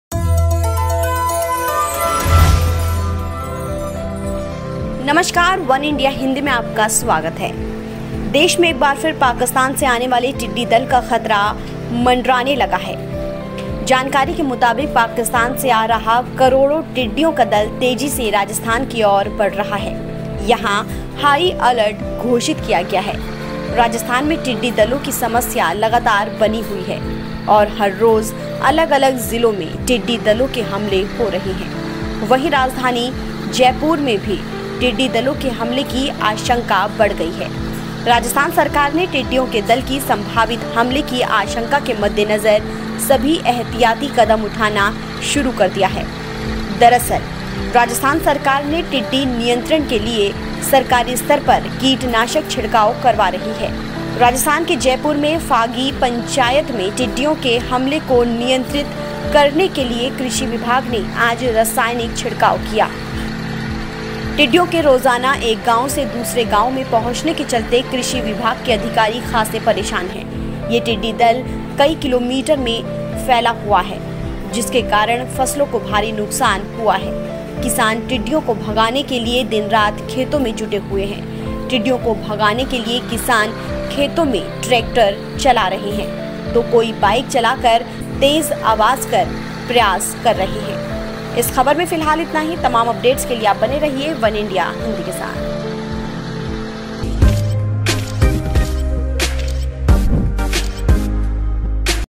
Locust Attack: राजस्थान में टिड्डियों को रोक ने के लिए किया जा रहा रासायनिक छिड़काव | वनइंडिया हिंदी
Rajasthan: Teams of Agriculture Department carry out chemical spraying in Phagi panchayat of Jaipur district to control the swarms of locusts, amid locust attack there.

राजस्थान में टिड्डी दलों की समस्या लगातार बनी हुई और हर रोज अलग-अलग जिलों में टिड्डी दलों के हमले हो रहे हैं. वंही राजधानी जयपुर में भी टिड्डी दलों के हमले की आशंका बढ़ गई है. राजस्थान सरकार ने टिड्डियों के दल की संभावित हमले की आशंका के मद्देनज़र सभी ऐहतियाती कदम उठाना शुरू कर दिया है